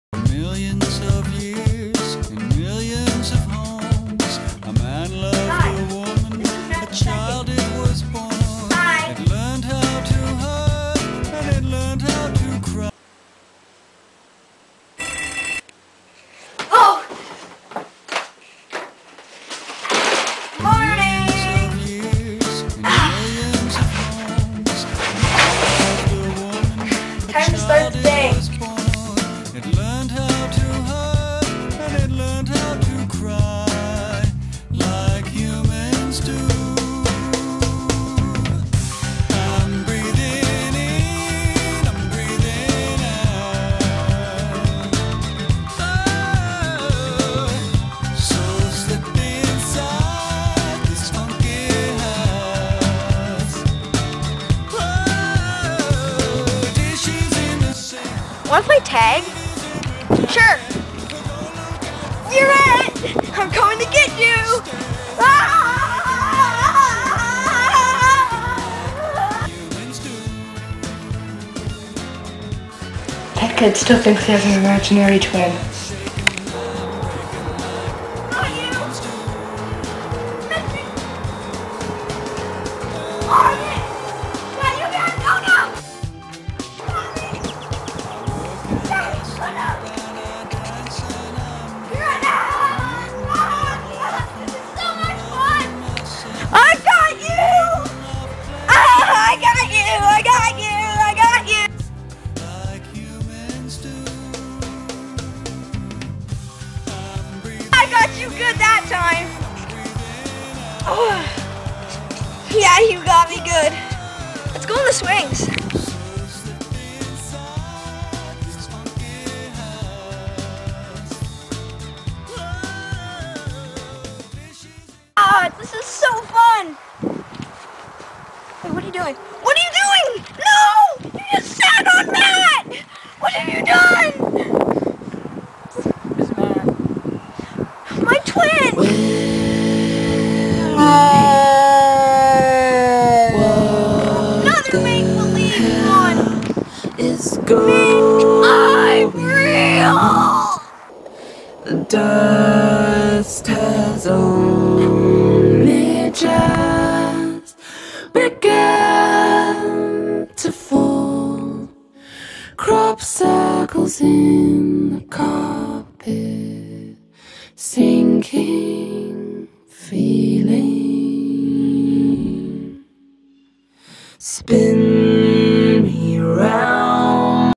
Mdog364- Twins
Matt gets a Imaginary friend who he thinks is real. find out the DRAMATIC trouth about his IMAGINARY friend.!?!!